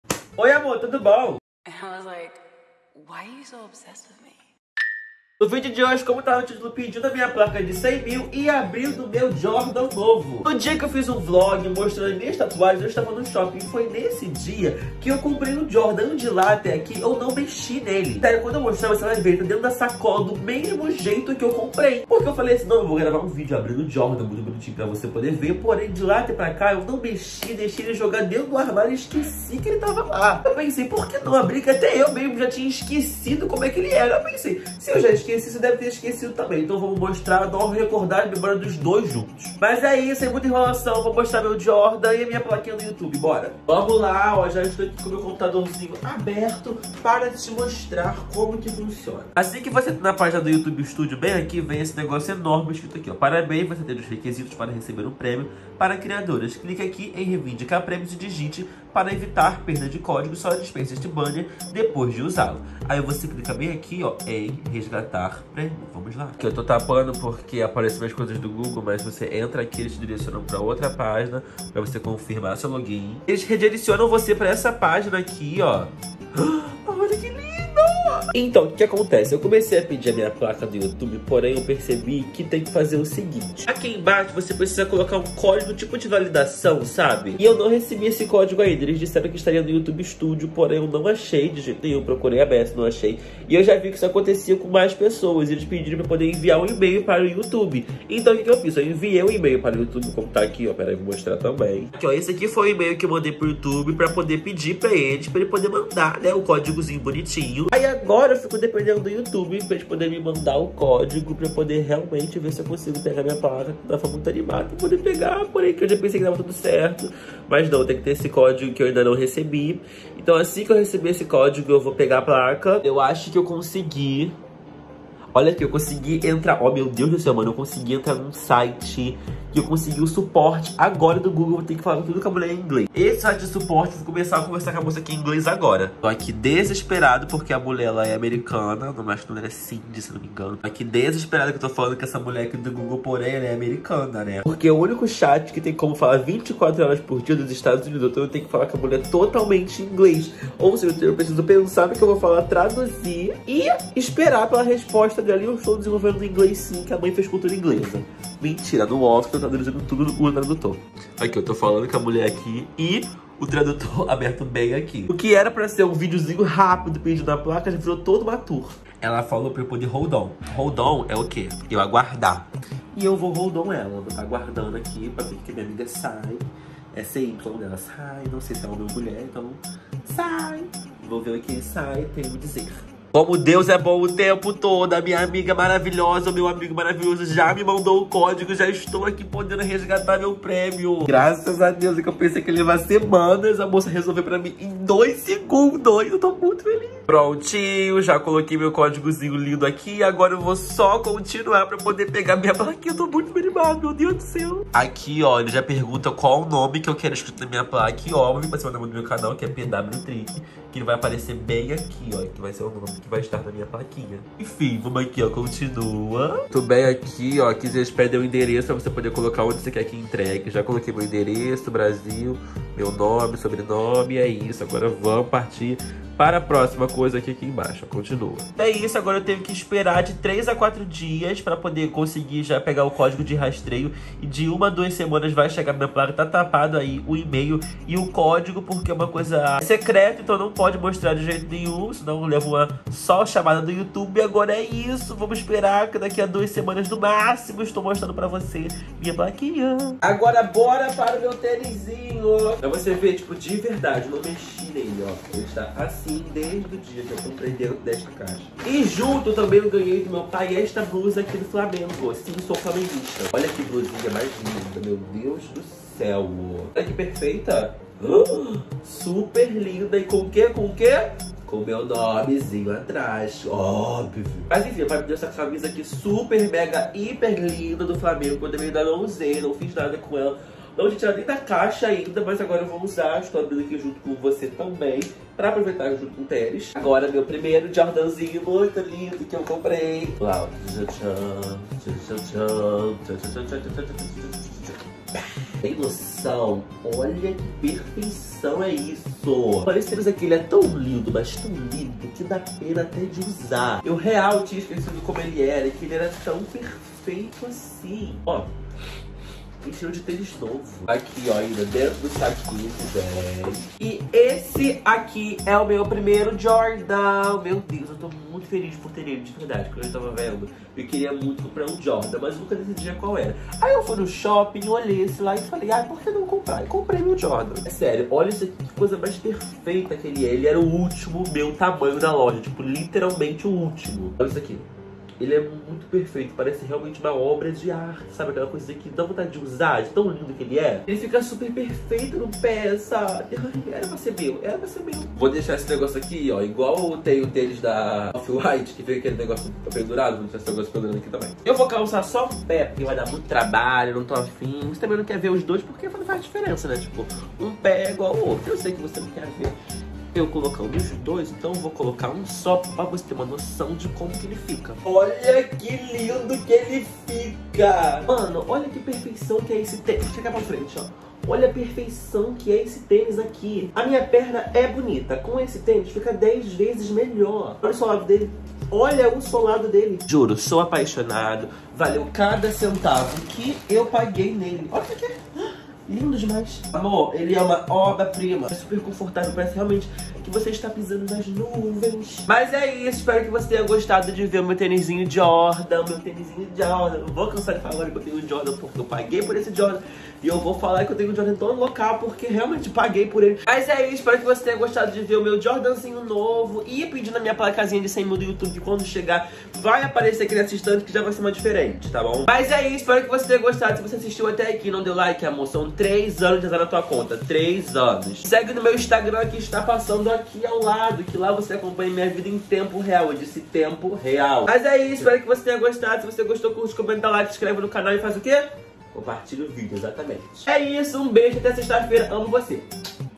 💖PEDINDO MINHA PLACA DE 100 MIL INSCRITOS + ABRINDO MEU JORDAN DE R$ 17.000 MIL REAIS 💸👟
Você me parece perdido então se inscreve aqui :)

Se você leu até aqui saiba q eu te amo MUITO e sou grato pela sua vida🤍✨